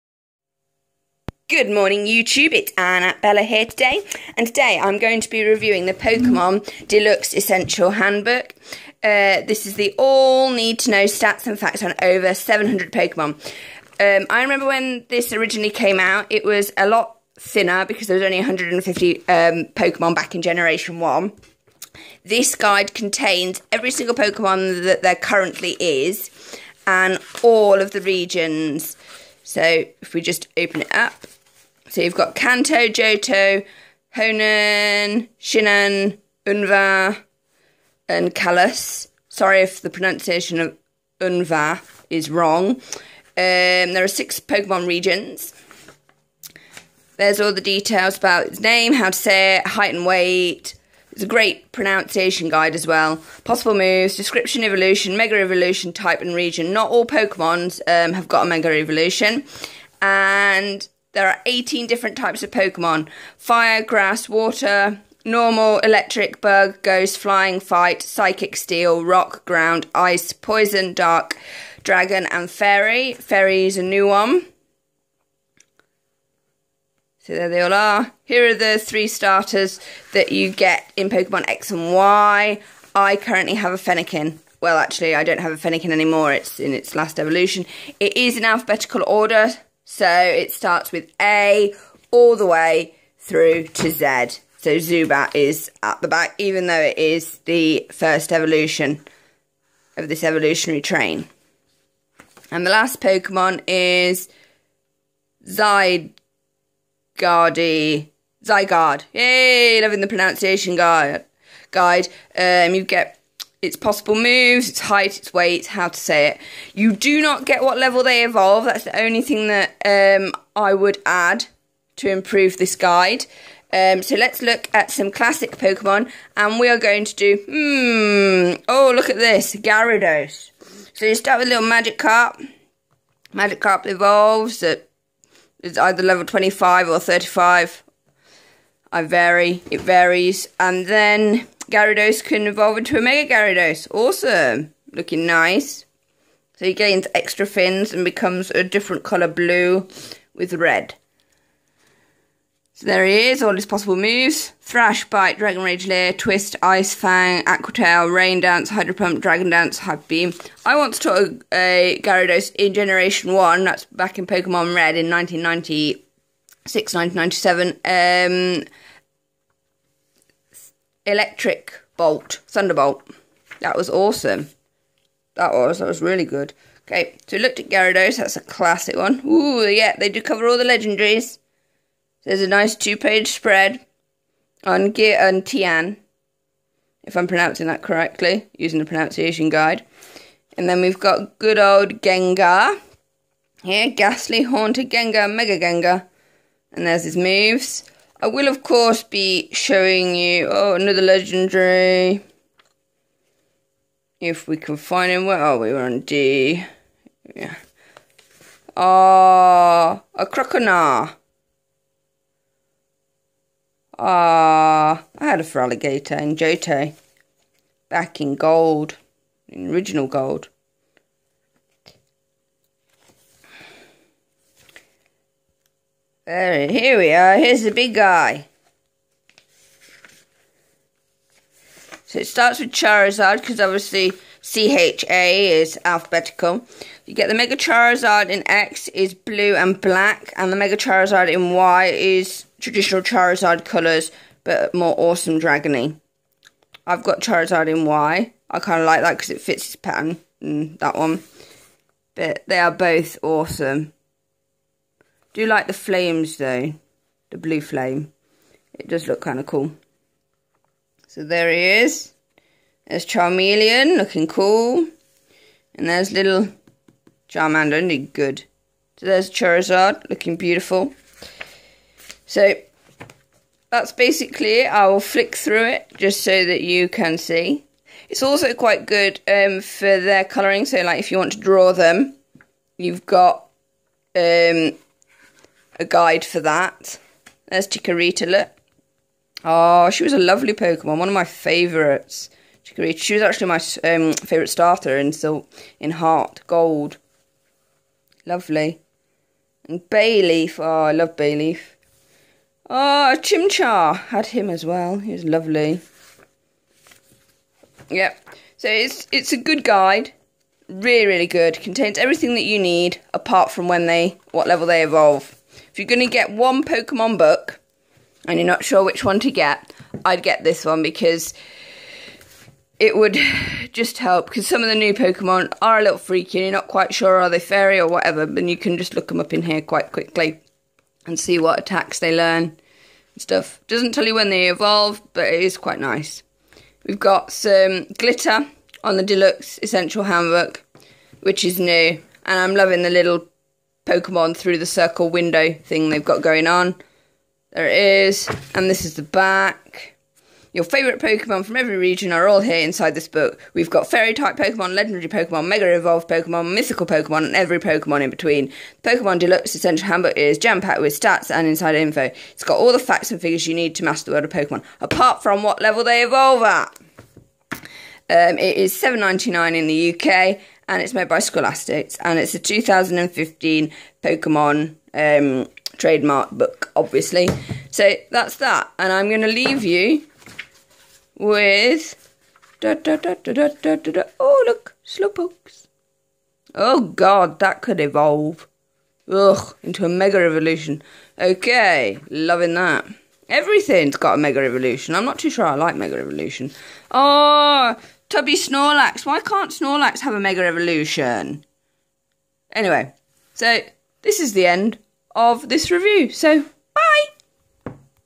Pokemon Deluxe Essential Handbook Review
My Pokemon Deluxe Essential Handbook (2015) Review featuring all 6 Regions and all the stats & facts on over 700 Pokemon as you 'Gotta Catch them all'. All your favourite Pokemon are here!